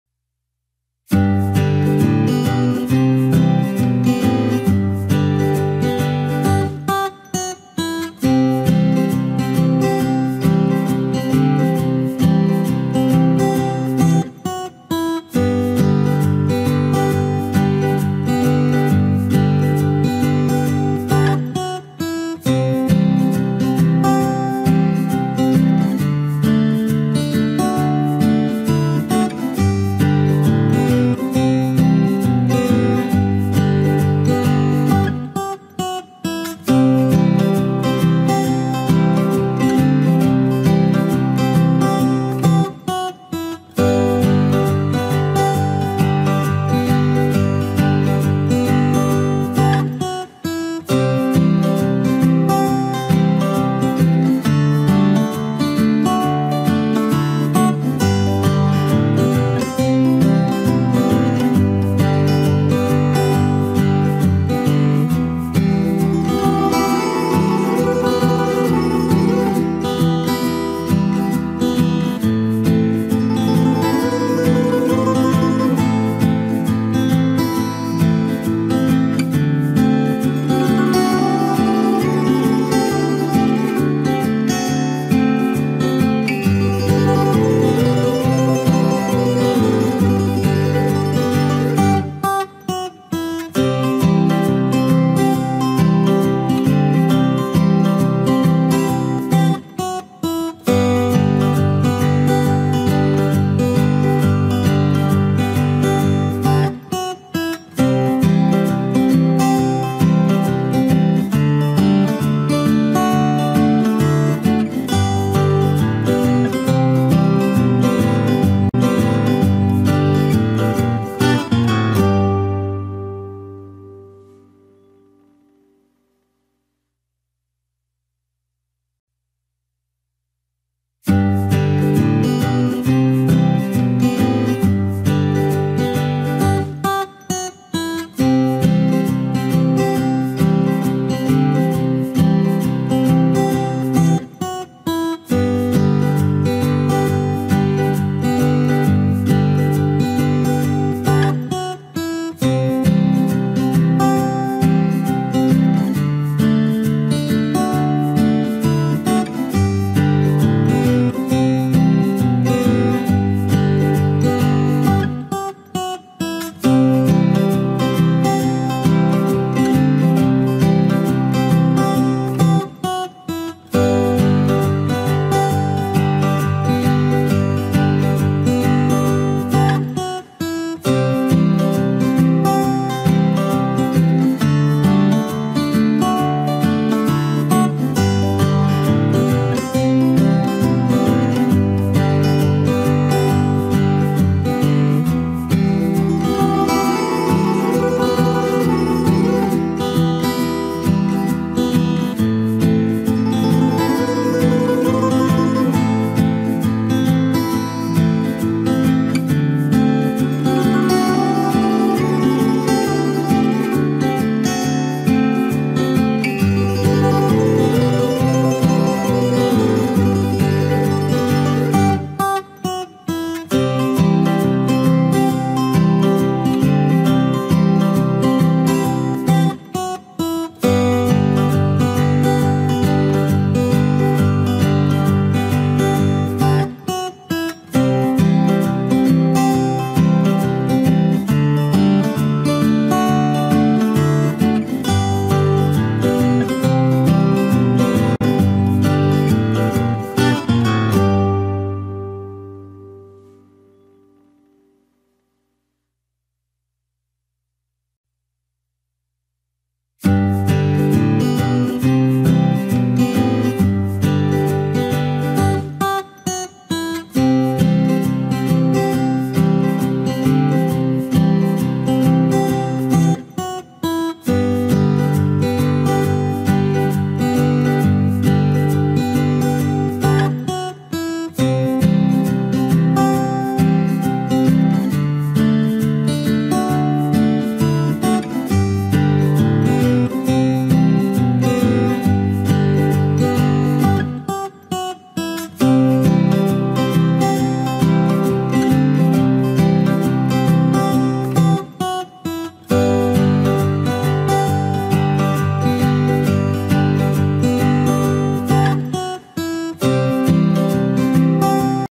John F. Kennedy Speed Painting in Adobe Photoshop (Digital Painting)
John Fitzgerald Kennedy (May 29, 1917 – November 22, 1963), commonly known as "Jack" or by his initials JFK, was the 35th President of the United States. He was in office from 1961 until his assassination in 1963. He was the youngest President elected to the office, at the age of 43. Events during his presidency included the Bay of Pigs Invasion, the Cuban Missile Crisis, the building of the Berlin Wall, the Space Race, the African American Civil Rights Movement, and early stages of the Vietnam War. He was the youngest President of the United States to die in office.

John F. Kennedy/Quotes

And so, my fellow Americans, ask not what your country can do for you -- ask what you can do for your country.

Change is the law of life. And those who look only to the past or present are certain to miss the future.

A rising tide lifts all boats

tanartstudio

🎵Track Info ▼

© Licence ▼

No need to add any kind of attribution in video description.

It does not represent their interests, nor have any connection with their activities*